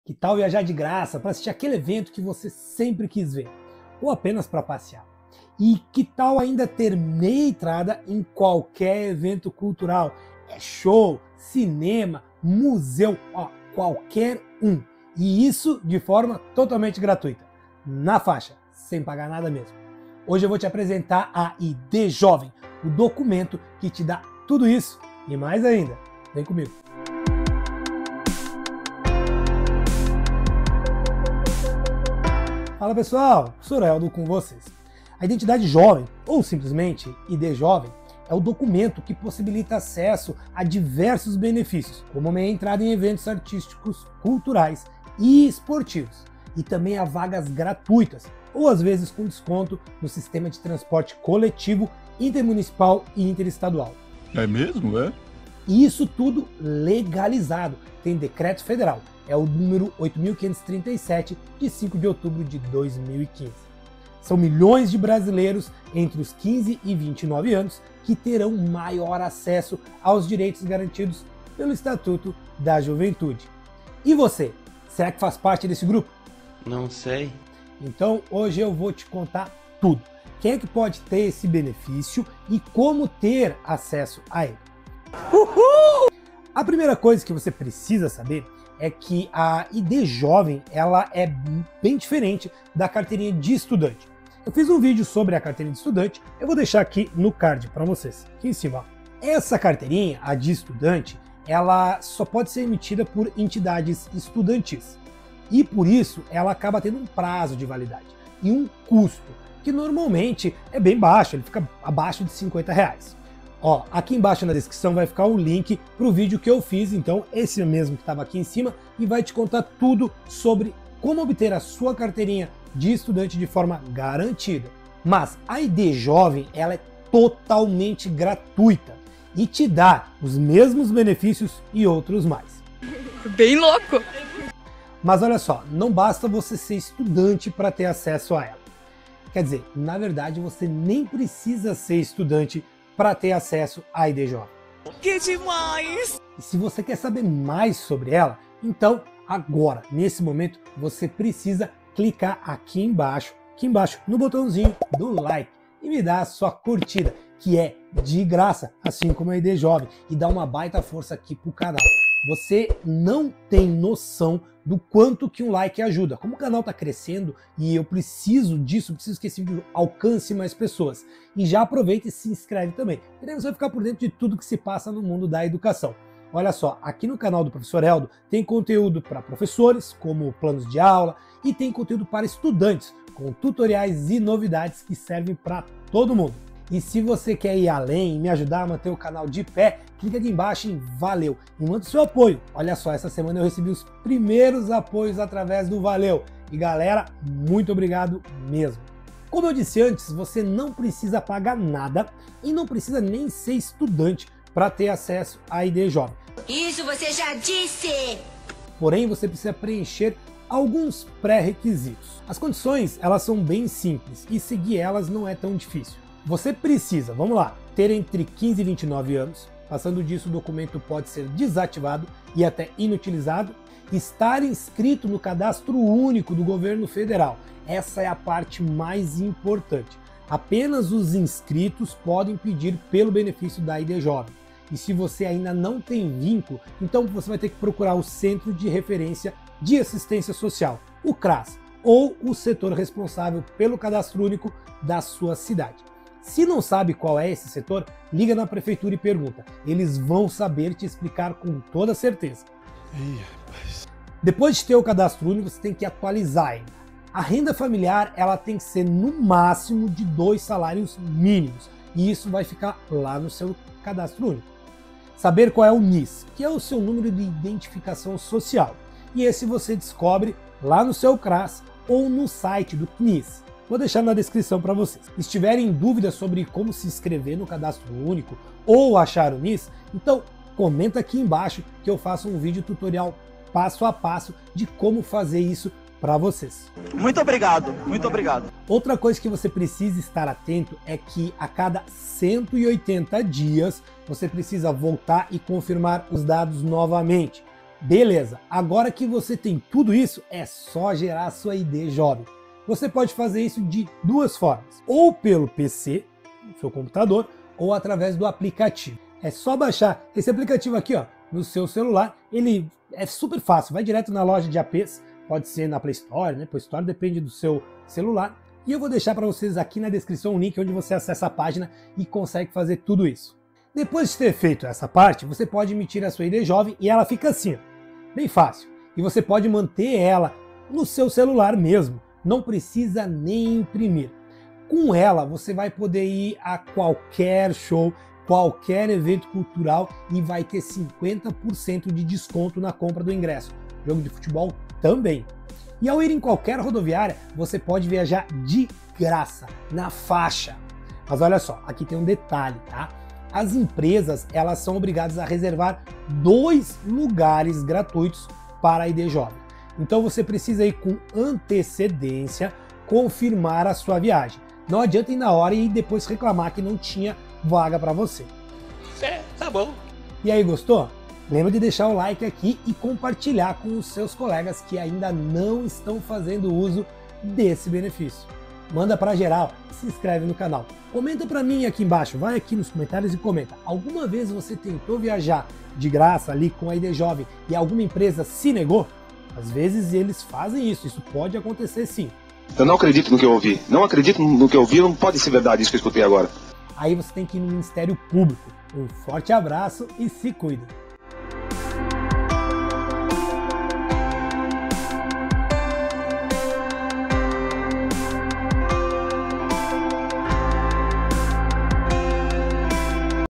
Como Conseguir sua ID JOVEM | Viajar de Graça | Meia Entrada
Que tal viajar de graça para assistir aquele evento que você sempre quis ver? Ou apenas para passear? E ter meia entrada para qualquer evento cultural, show, cinema, museu, em fim qualquer um, e isso de forma gratuita, na faixa sem pagar nada mesmo? Hoje eu vou te apresentar a ID Jovem, o documento que te dá tudo isso e mais. VEM COMIGO!

👉🏻CURSO PROJETO DE VIDA NA PRÁTICA PARA O NOVO ENSINO MÉDIO